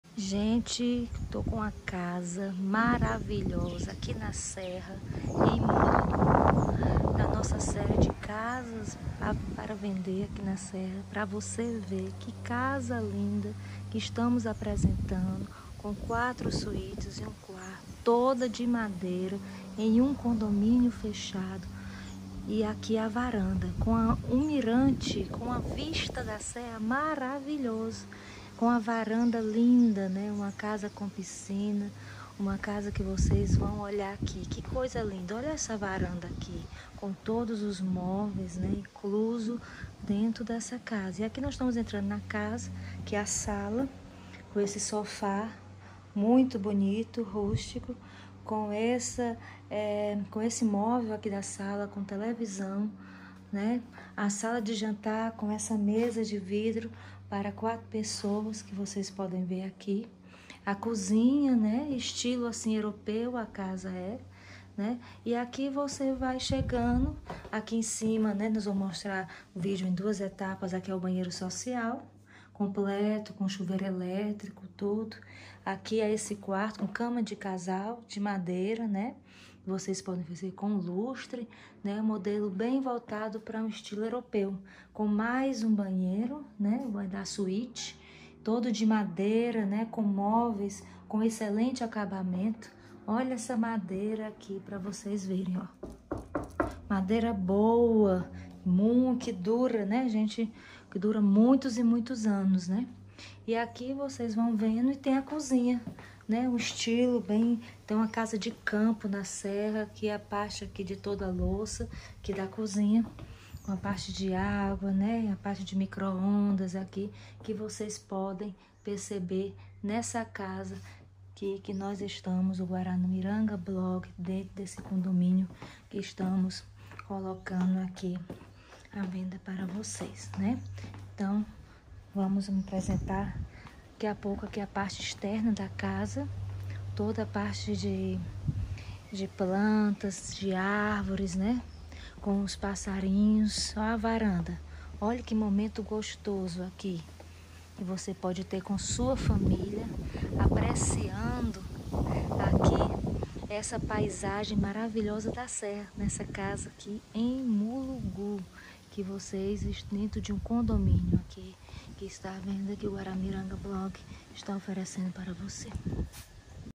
Maravilhosa Casa em Condomínio Mobiliada com 05 quartos em Guaramiranga
Casa em condomínio de alto padrão à venda em Guaramiranga com 04 suítes + 01 quarto, sala de estar, cozinha, churrasqueira, piscina térmica, lareira, jacuzzi. 450m2 de área construída e 04 hectares de terreno. Vista maravilhosa da serra! Venda com todos os móveis inclusos.

Tem uma vista fantástica para as montanhas por ser localizada no topo de uma delas e fica dentro de uma floresta nativa muito bem preservada.

A casa fica em um condomínio de quatro residências, no alto da montanha, com vista espetacular para as serras envolvida pela Mata Atlântica preservada.

A casa foi construída em harmonia com toda essa natureza e tem fácil acesso por rodovias asfaltadas.